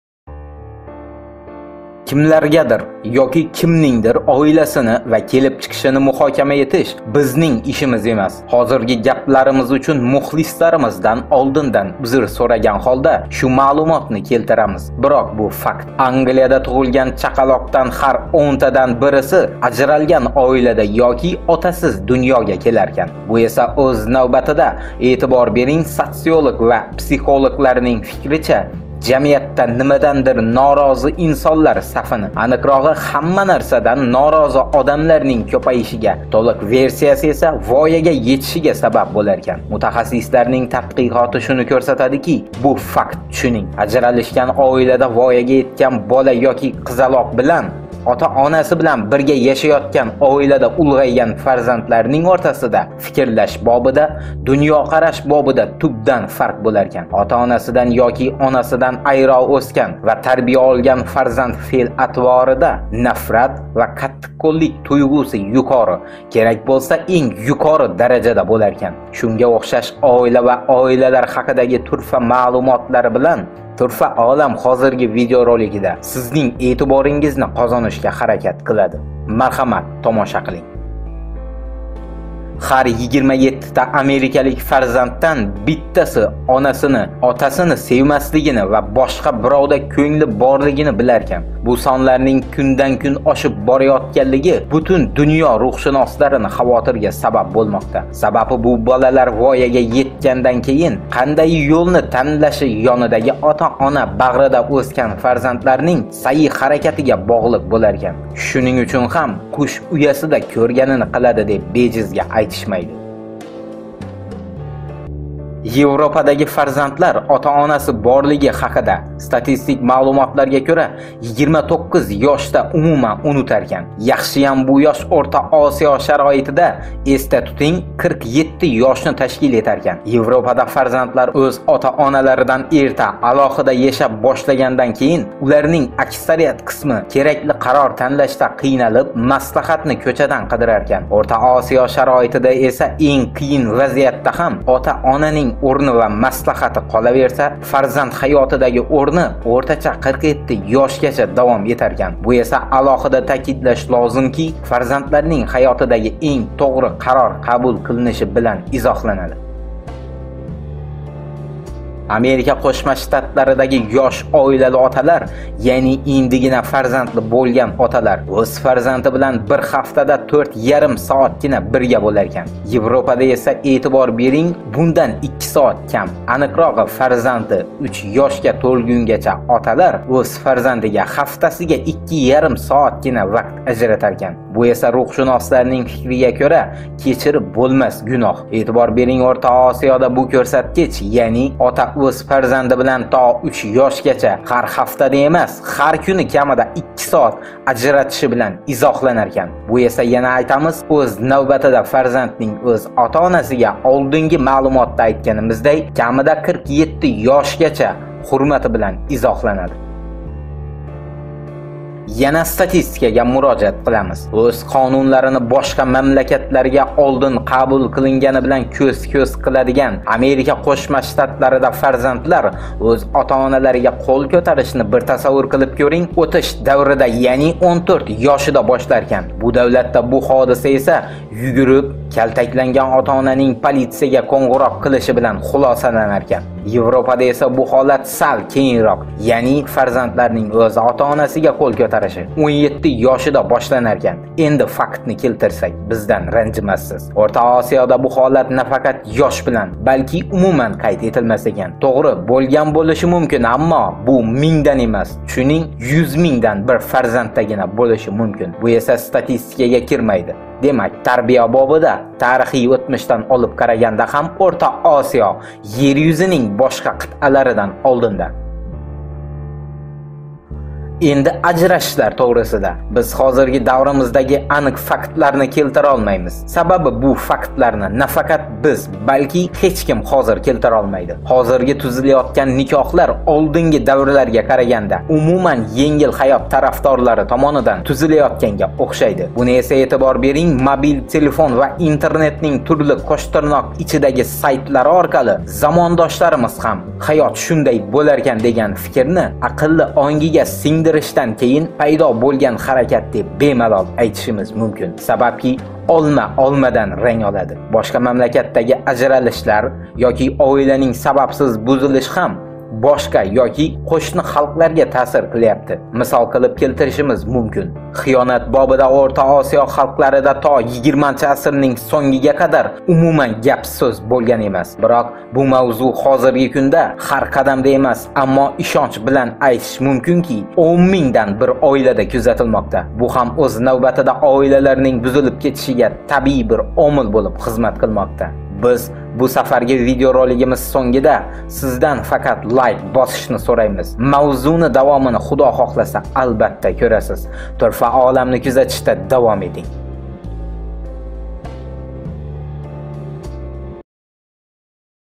ОИЛА ХАКИДА СИЗ СУРАГАН ЭНГ КИЗИК МАЪЛУМОТЛАР ТОП--5
Мутахасисиларнинг тадкикоти шуни курсатадики бу факт
тушунинг ажрашилган оилада вояга етган бола еки кизалок билан бирга яшаетган оилада улгайган фарзандлар фикрлаш
боида яъни дунекараш бобида тубдан фарк  киларкан. Отасидан еки онасидан айро усган ва тарбия олган фарзанд феъл атворида нафрат ва каттиккуллик туйгуси юкори керак булса энг юкори даражада буларкан.
Шунга ухшаш оила ва оилалар хакидаги турфа маълумотлари билан Турфа Олам хозирги видеороликда сизни эътиборингизни козонишга харакат килади.Мархамат тамоша килинг.

Произведение «ОИЛА ХАКИДА СИЗ СУРАГАН ЭНГ КИЗИК МАЪЛУМОТЛАР ТОП--5» созданное автором по имени AKATOR, публикуется на условиях лицензии Creative Commons «Attribution» («Атрибуция») 4.0 Всемирная.